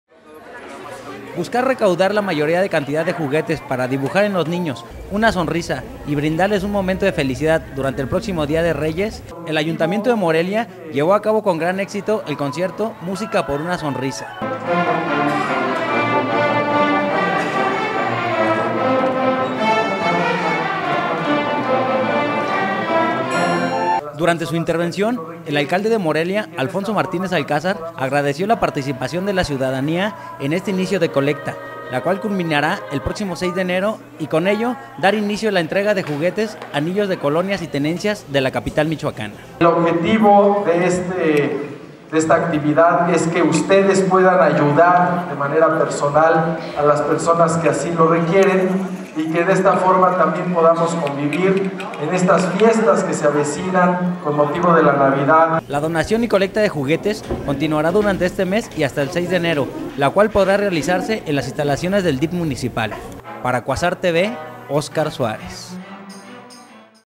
Música por una Sonrisa
Buscan recaudar la mayor cantidad de juguetes para dibujar en los niños una sonrisa y brindarles un momento de felicidad durante el próximo Día de Reyes y el Ayuntamiento de Morelia, llevó a cabo con gran éxito el concierto "Música por una Sonrisa"...